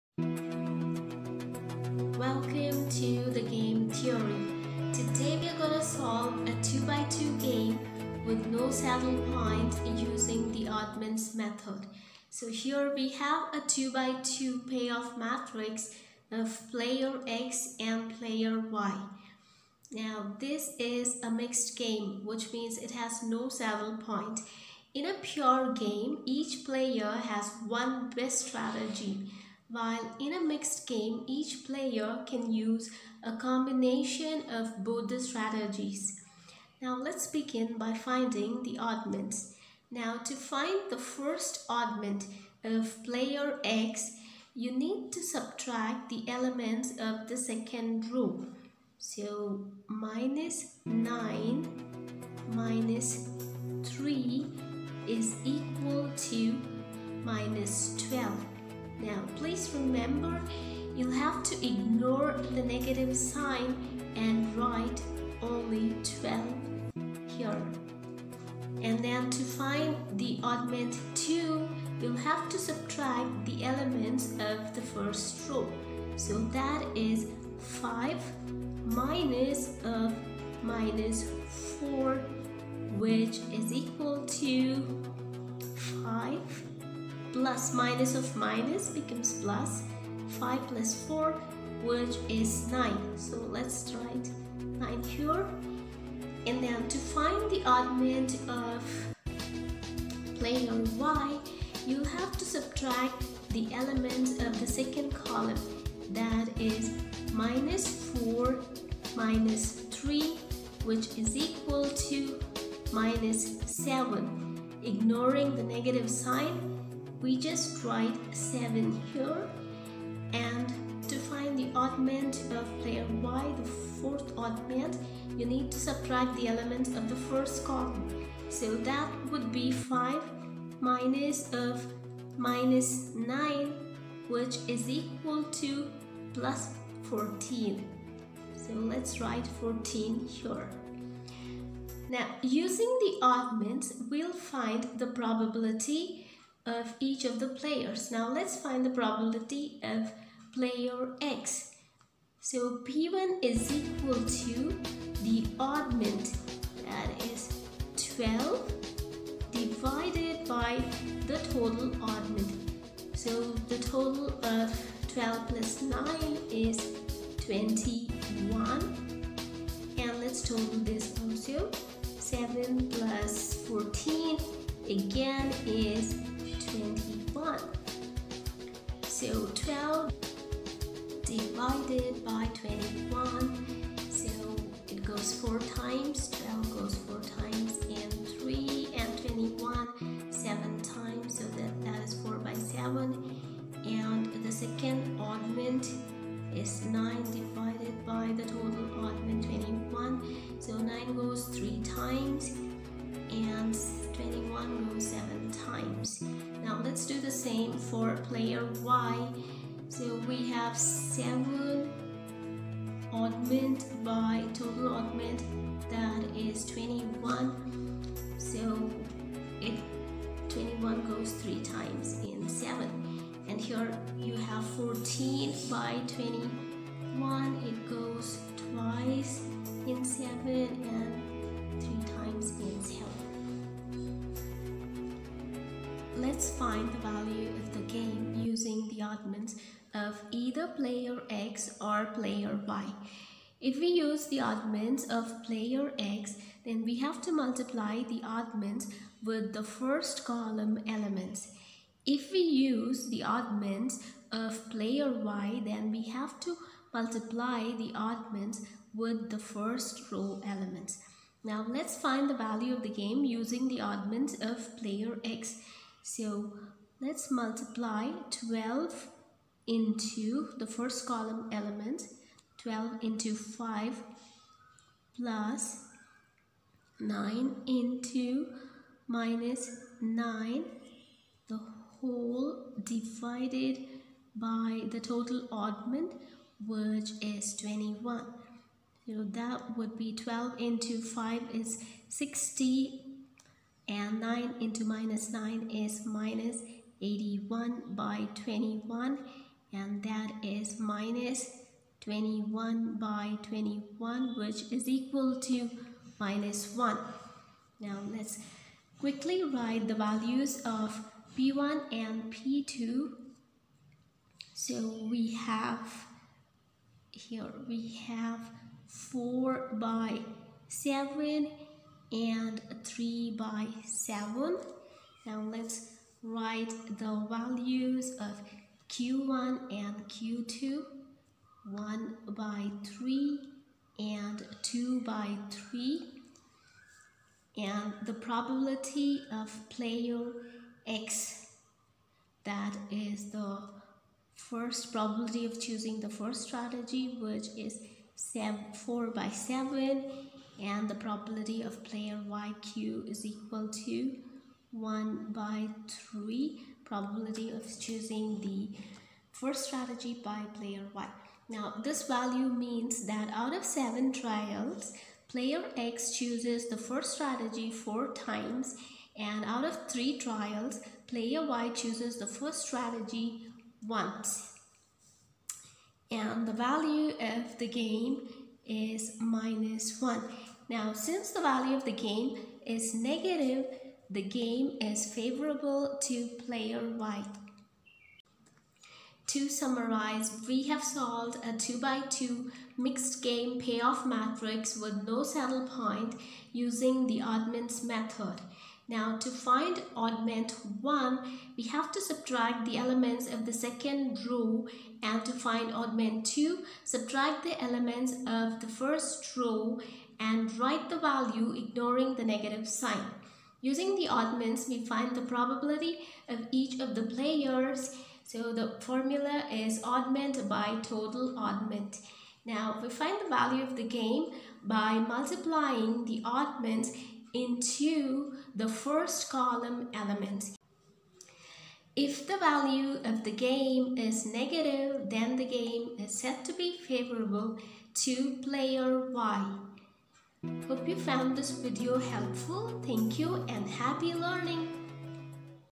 Game Theory Series #9 | Problem 2 - Oddments Method
Operations Research Management
Game Theory Problem
Oddments Method
V = Negative